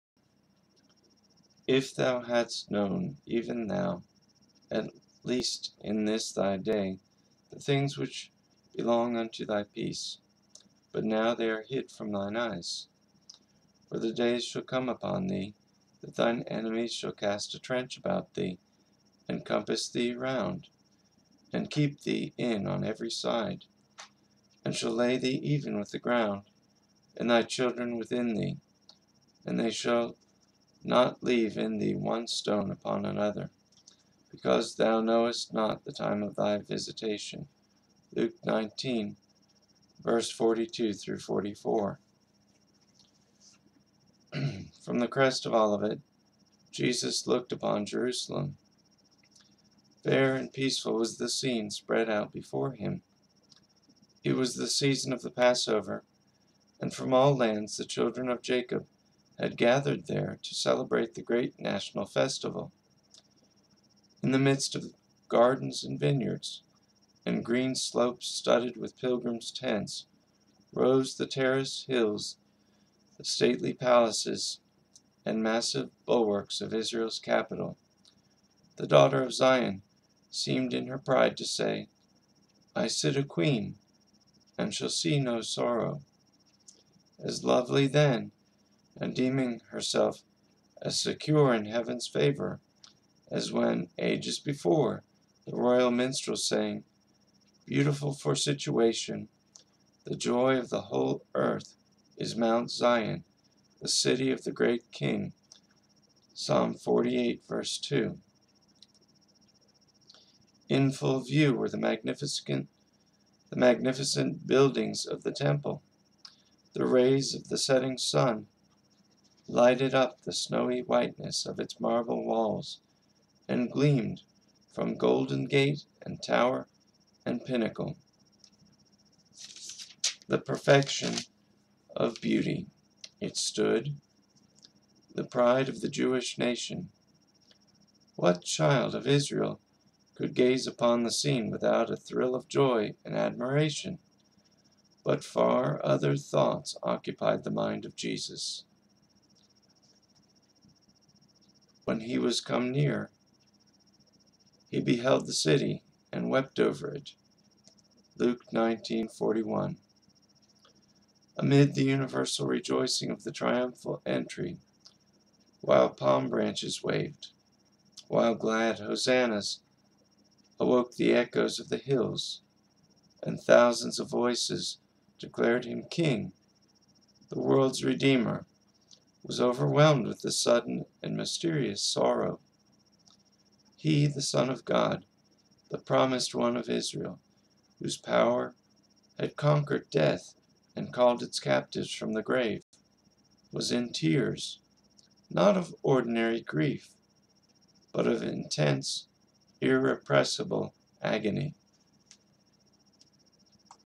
The World's Redeemer, Overwhelmed With Sorrow
Great Controversy, chapter 1, part 1, "The Destruction of Jerusalem",
someone reading The Great Controversy

Luke 19:42-44
Psalm 48:2
Luke 19:41